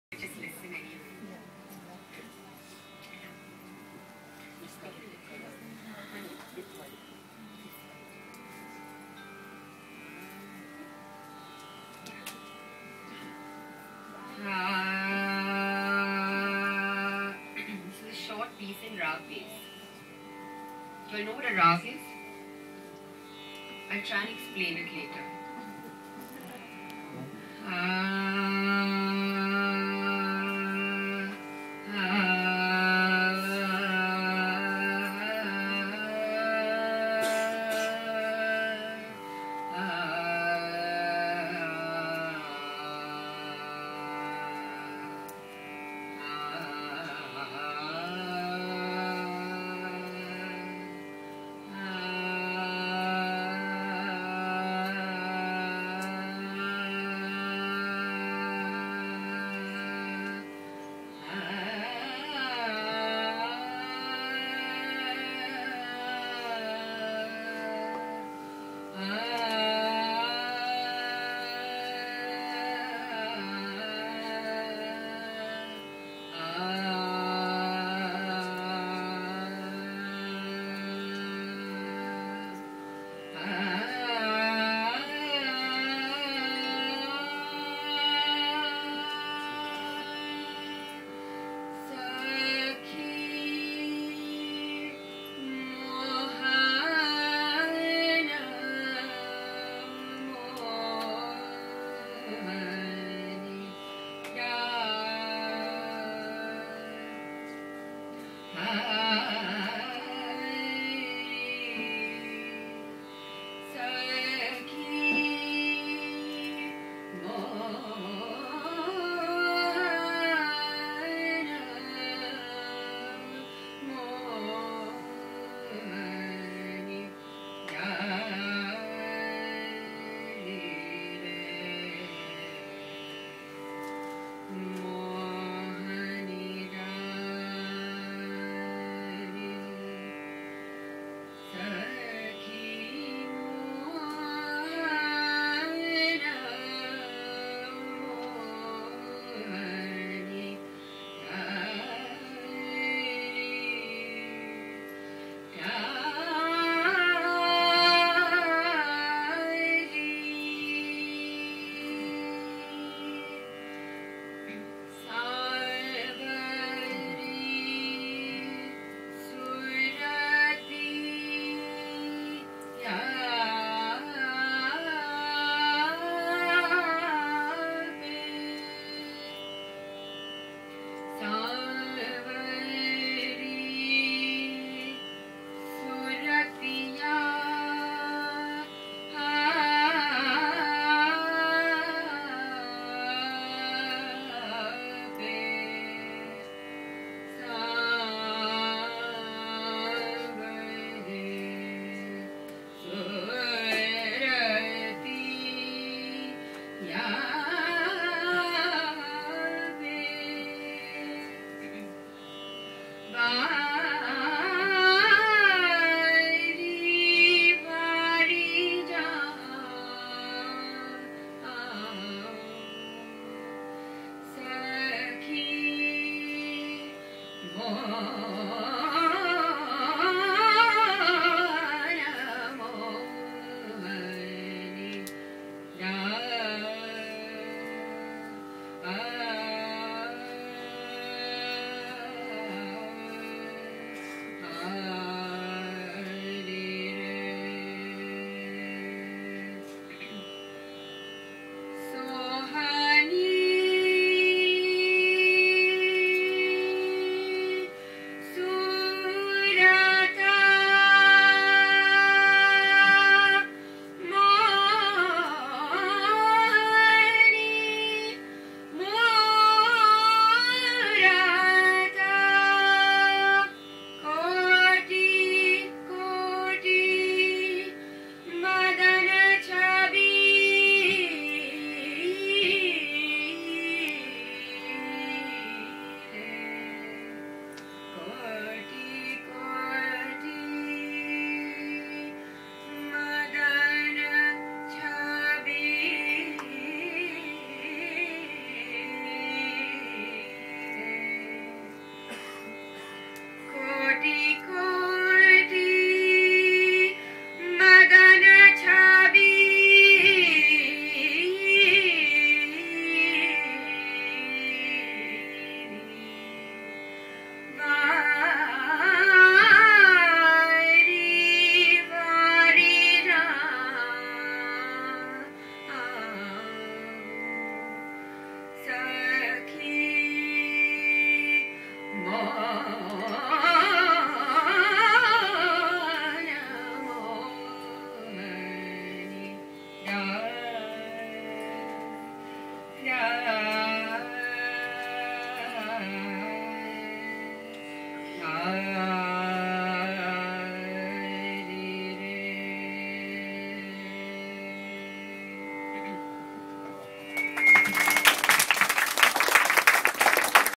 The Music Room At One Up - Video 2 of 6
Book reading & a discussion of "The Music Room" By Namita Devidayal at One Up Library, Book Store & Experience Centre Amritsar on the 3rd of September 2011